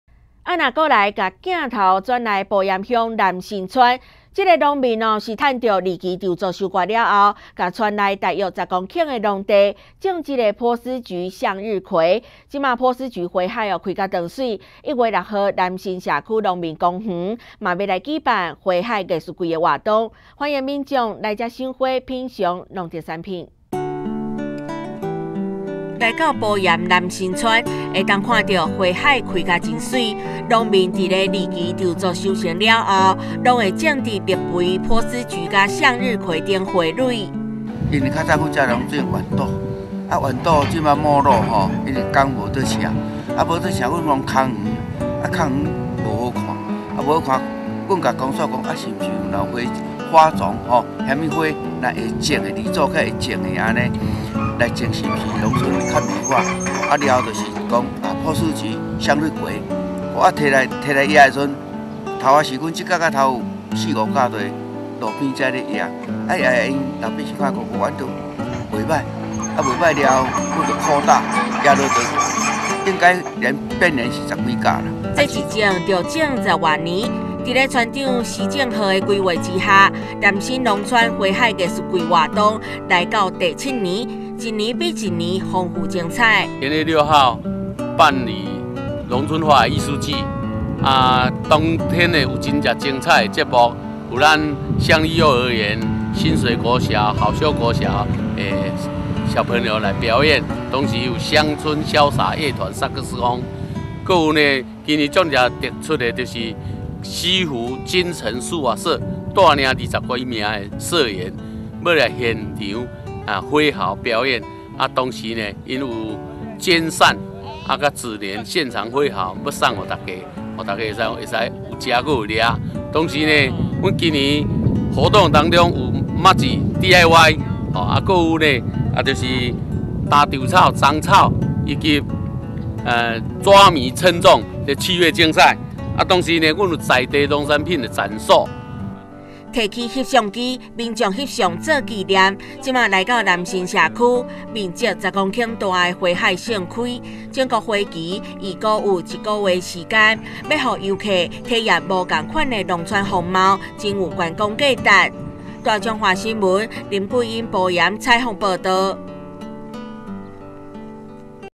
1070102休耕地種波斯菊 花海綻放美不勝收
趁著第二期稻作收割完畢後，埔鹽鄉南新村為了活用休耕的農地，把村內大約十公頃的農地，種波斯菊、向日葵，現在波斯菊花正盛開，1月6日南新社區農民公園將舉辦花海藝術季活動，歡迎民眾來這賞花、品嘗農特產品。